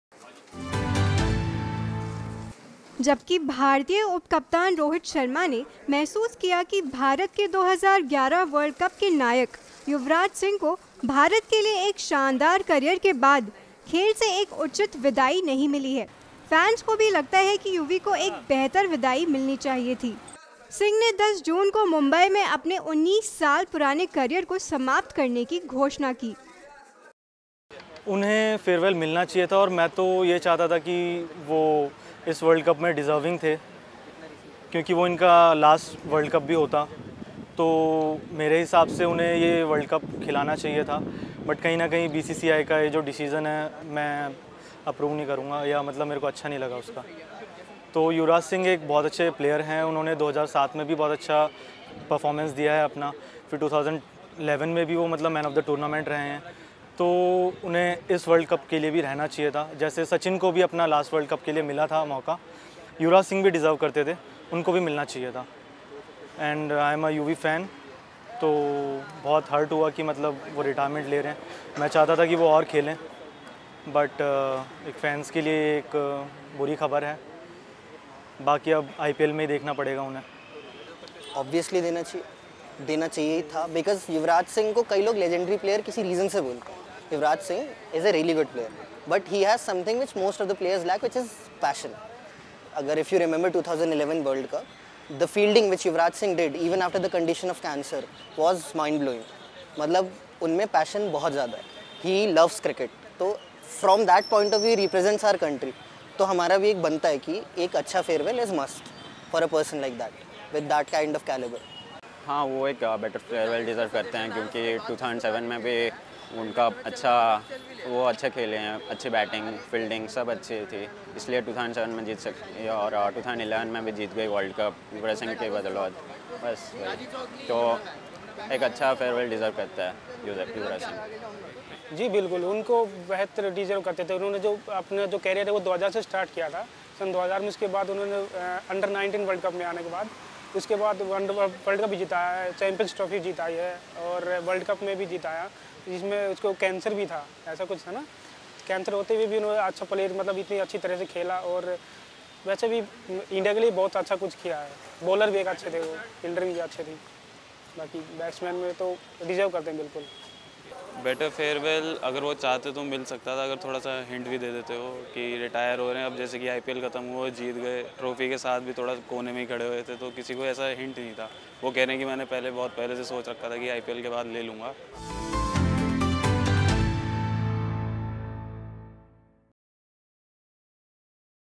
फैंस को लगता है युवराज सिंह प्रॉपर विदाई डिजर्व करते थे
जबकि भारतीय उप-कप्तान रोहित शर्मा ने महसूस किया कि भारत के 2011 वर्ल्ड  कप के नायक युवराज सिंह को भारत के लिए एक शानदार करियर के बाद खेल से एक उचित विदाई नहीं मिली है, फैंस को भी लगता है कि युवी को एक बेहतर विदाई मिलनी चाहिए थी।